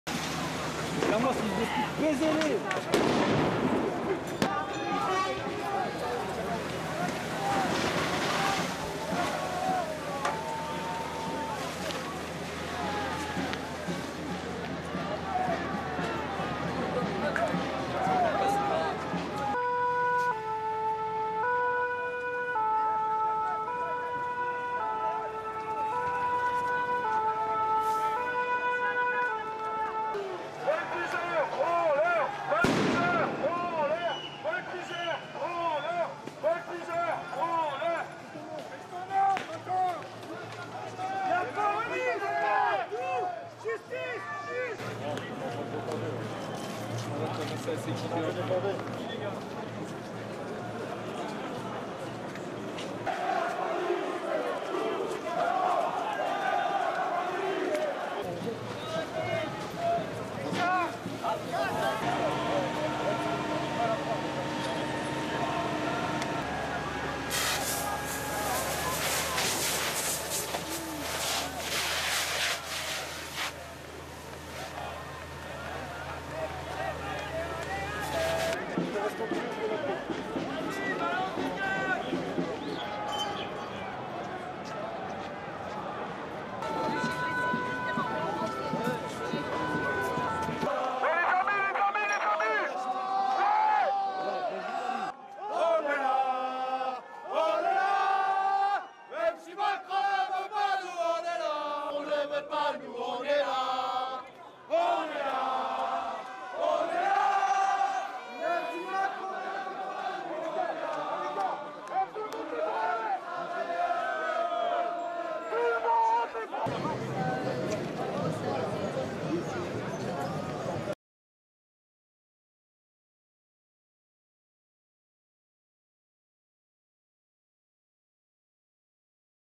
Police fire tear gas at crowds on the one year anniversary of the yellow vest protests
Several arrests have been made as police use tear gas and water cannons against protesters in the streets of Paris, France.

On the one year anniversary of the yellow vest protesters, hundreds of people have taken to the streets of Paris in a mass demonstration.